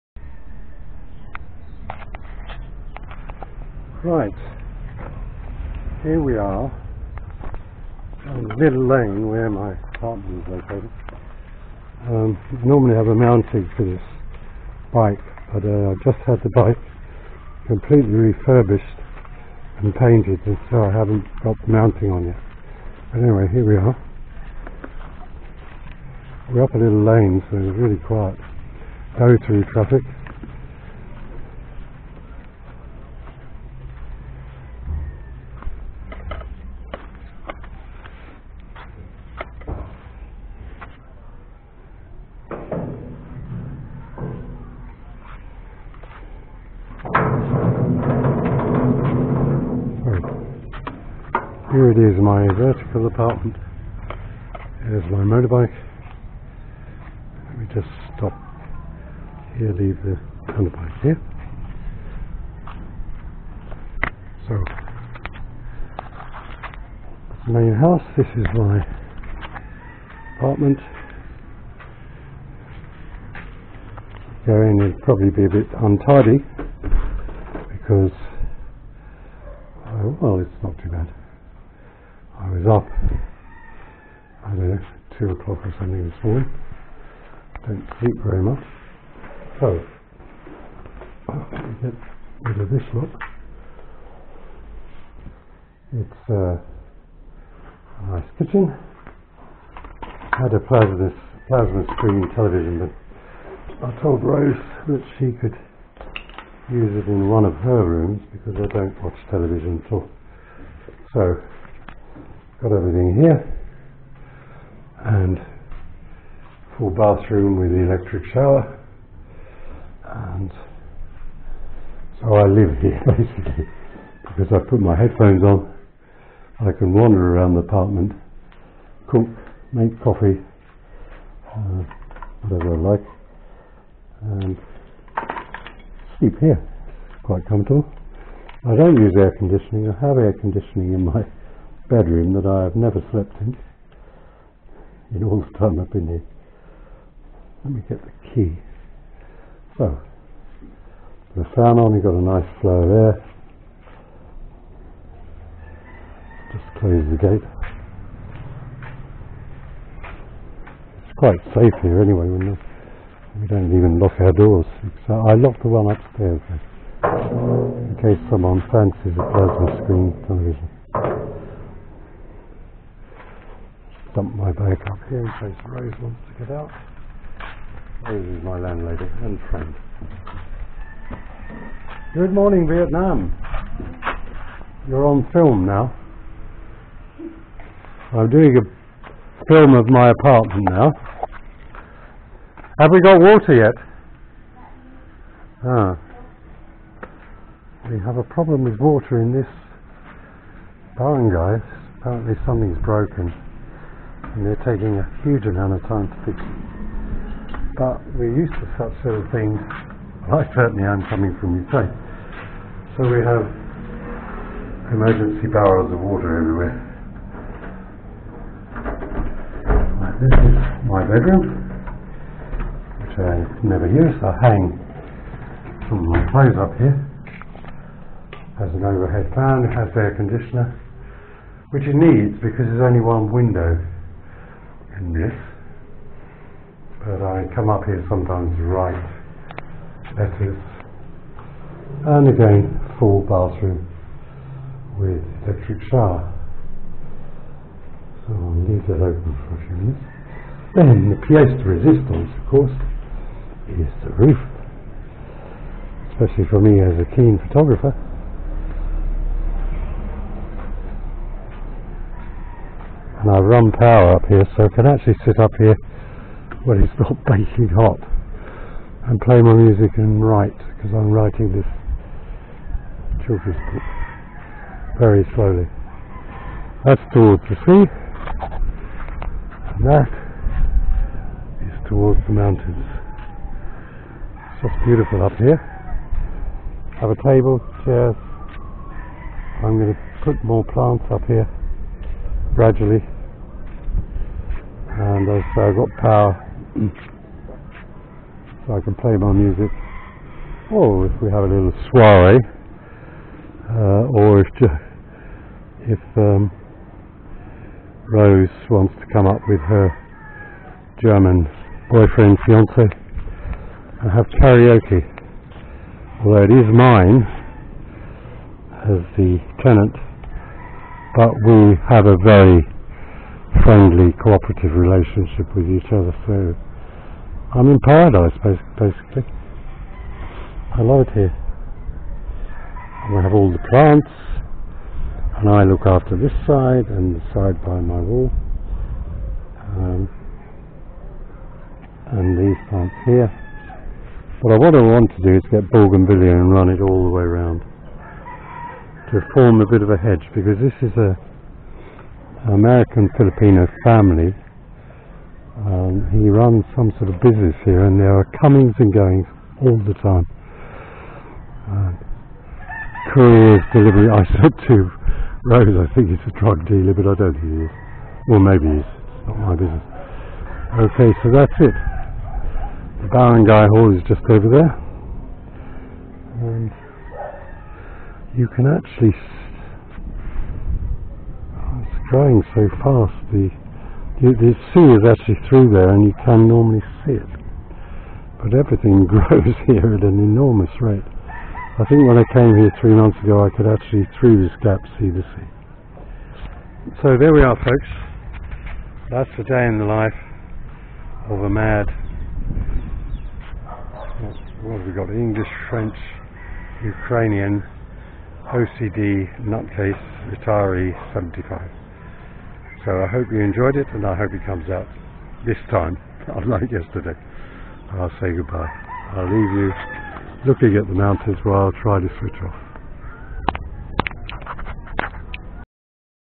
Talking on the beach at sunrise 7th and last.
Bonus special; a quick tour of my vertical apartment and roof garden.